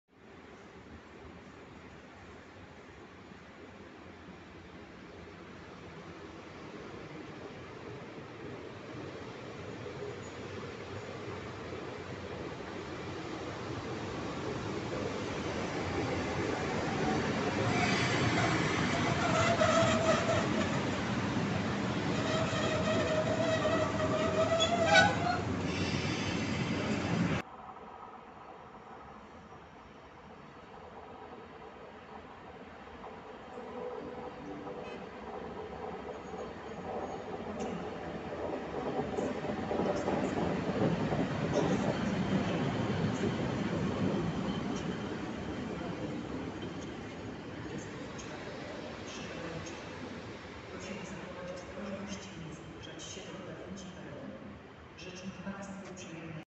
Manewry lokomotywy EP07-534 na stacji Kraków Główny.
Pozdrawiam maszynistę.